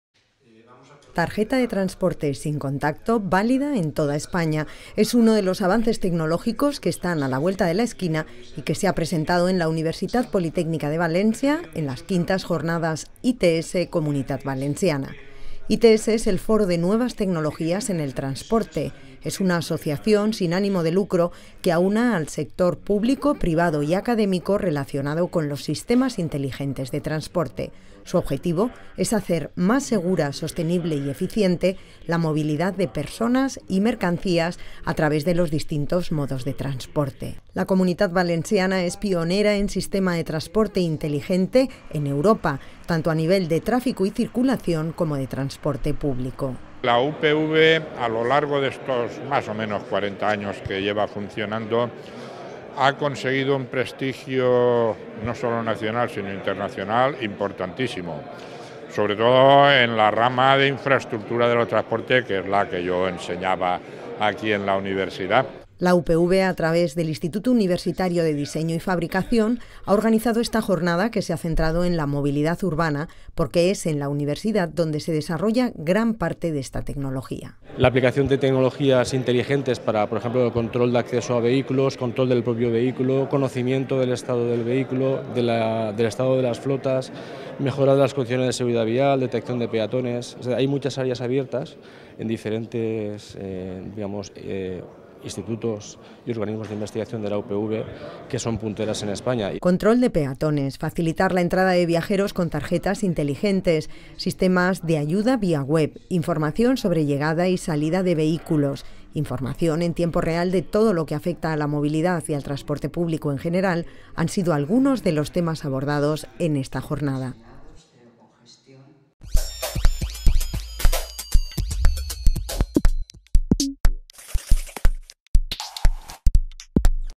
Jornada ITS Comunitat Valenciana - Noticia @UPVTV 07-03-2016
Tarjeta de transporte sin contacto válida en toda España. Es uno de los avances tecnológicos que están a la vuelta de la esquina y que se ha presentado en la Universitat Politècnica de València, en las V Jornadas ITS Comunitat Valenciana.

ITS es el Foro de Nuevas Tecnologías en el Transporte. Es una Asociación sin ánimo de lucro que aúna al sector público, privado y académico relacionado con los Sistemas Inteligentes de Transporte. Su objetivo es hacer más segura, sostenible y eficiente la movilidad de personas y mercancías, a través de los distintos modos de transporte.

La Comunitat Valenciana es pionera en sistema de transporte inteligente, en Europa, tanto a nivel de tráfico y circulación como de transporte público.

La UPV, a través del Instituto Universitario de Diseño y Fabricación, ha organizado esta jornada que se ha centrado en la movilidad urbana, porque es en la universidad donde se desarrolla gran parte de esta tecnología.

Control de peatones, facilitar la entrada de viajeros con tarjetas inteligentes, sistemas de ayuda vía web, información sobre llegada y salida de vehículos, información en tiempo real de todo lo que afecta a la movilidad y al transporte público en general, han sido algunos de los temas abordados en esta jornada.